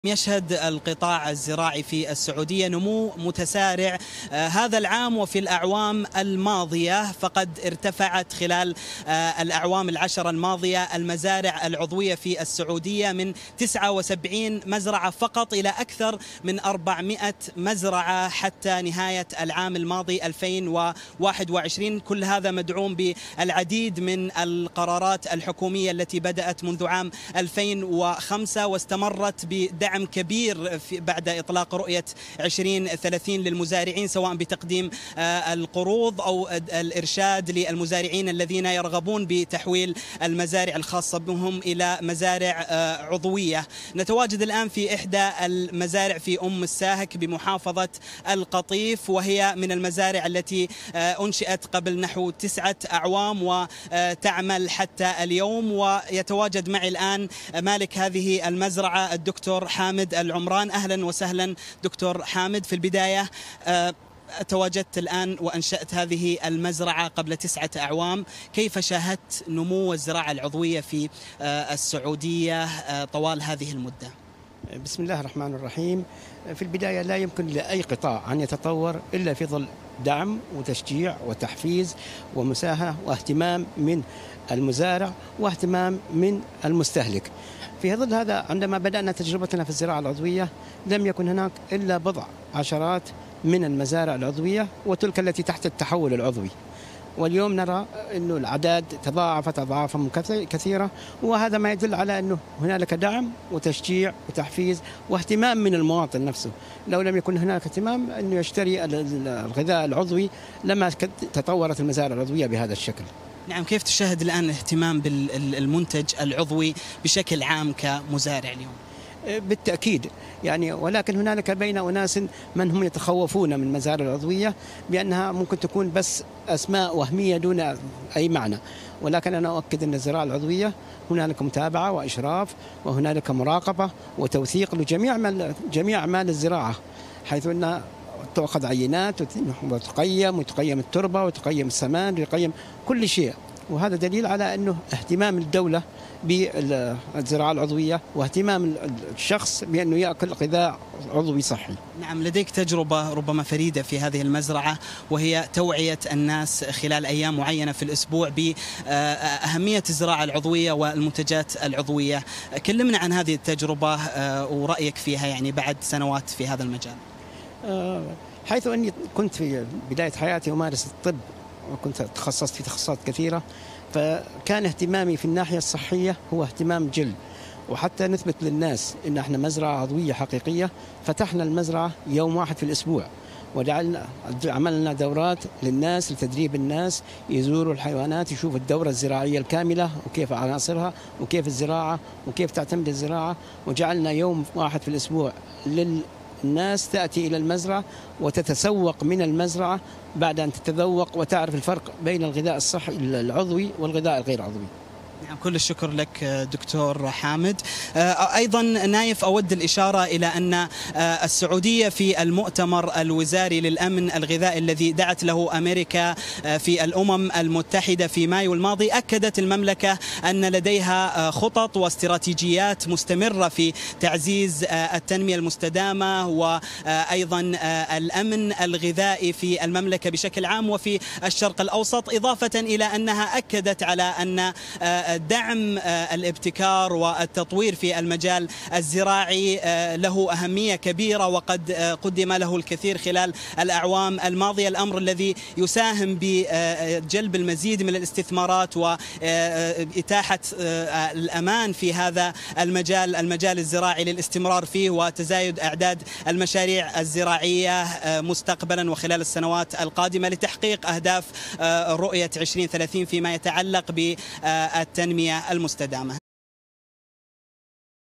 السعودية.. 400 مزرعة للمنتجات العضوية
نشرة الرابعة | أعلنت وزارة البيئة والمياه والزراعة في السعودية
عن ارتفاع نسبة الزراعات العضوية في البلاد إلى ما يصل لـ 265% خلال العشر سنوات الماضية يأتي ذلك في وقت ارتفعت أعداد المزارع في السعودية من 79 مزرعة إلى 400 مزرعة بنهاية 2021